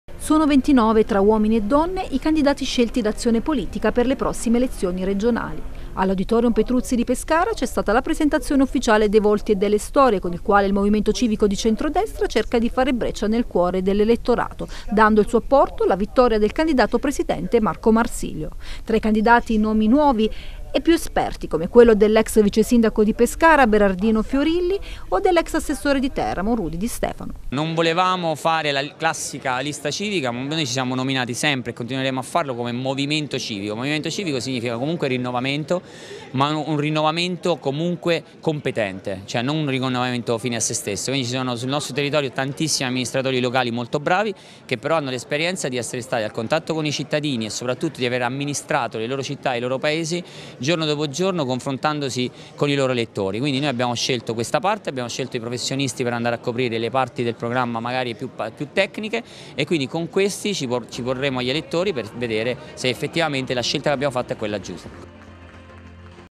Azione Politica presenta la squadra per le Regionali
Azione Politica ha presentato a Pescara le sue liste per le prossime regionali. 29 volti nuovi tra esperienza e rinnovamento